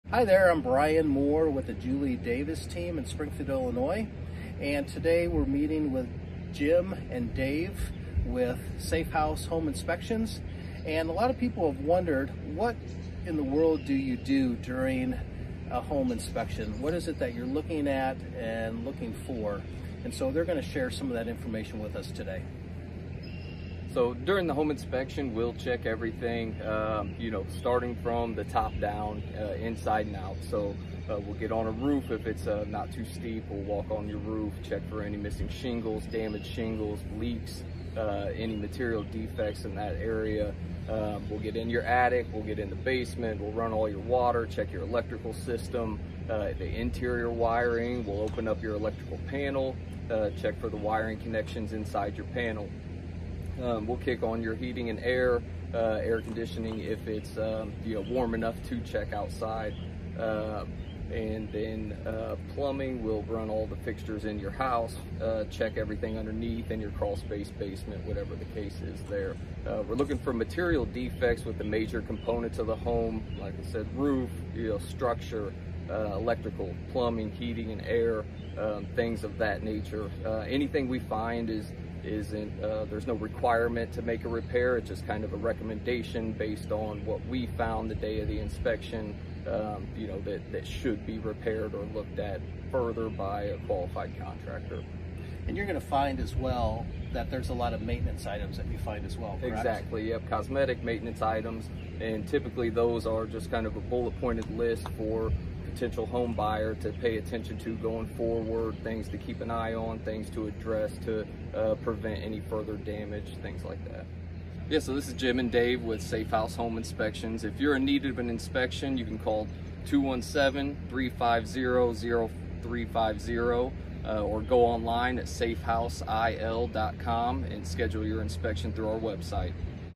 What is inspected during a home inspection
Brian is back with Jim and Dave from Safe House Home Inspections discussing what actually is inspected during a home inspection.